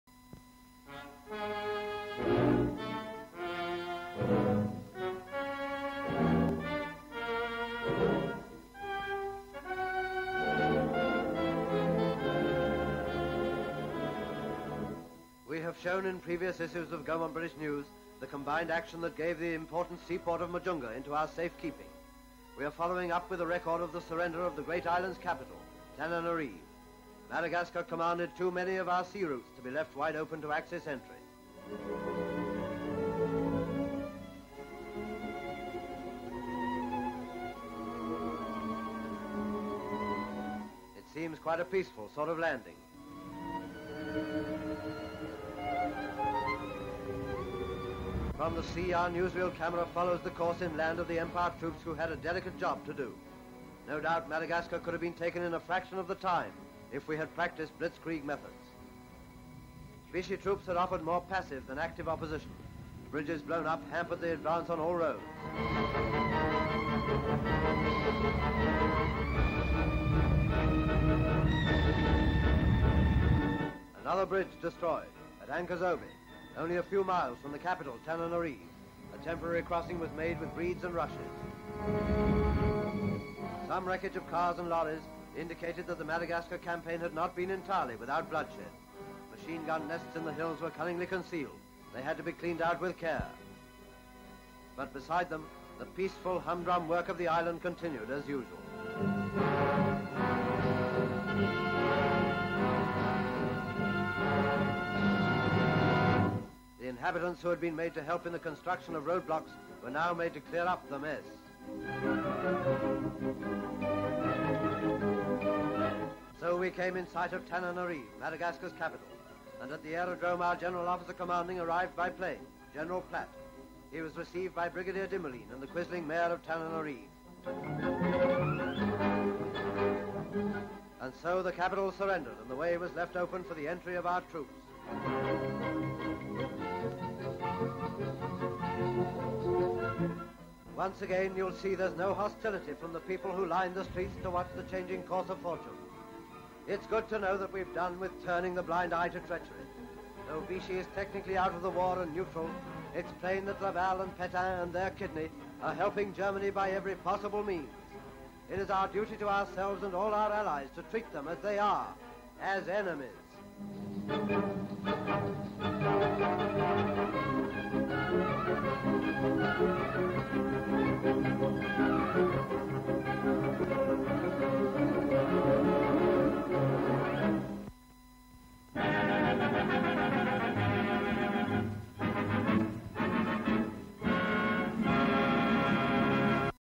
Invasion of Madagascar by British soldiers and sailors (1942)
British Army and Royal Navy forces invade Madagascar and seize it from Vichy France Axis units to protect shipping routes

Full Description:

SLATE INFORMATION: South African War Zone: British Troops Take Control in the Capital of Madagascar

MADAGASCAR: Various:
EXT

MADAGASCAR. Ships leave Diego Suarez..A.C.L's land Majunga..Tommy w. umbrella..blown up bridges..Gen. Platt meets Brig. Dimoline in Tananarive..troops march thro' city & native scene..(IN CUTS):- 3 King's African Rifles receive military medals for Gen. Sir W. Platt

MILITARY BRITISH 3 King's African Rifles received military medals from Gen. Sir W. Platt in Madagascar

NATIVES Natives when troops occupy Tananarive..woman washes..scenes in city (Madagascar)

PLATT, General. Decorates 3 King's African Riflemen in Madagascar

UMBRELLAS Tommy w. umbrella in Maagascar

UMBRELLAS Tommy w. umbrella in Catania (Sicily)

South Africa; Military - Active

Background: British Army and Royal Navy forces invade Madagascar and seize it from Vichy France Axis units to protect shipping routes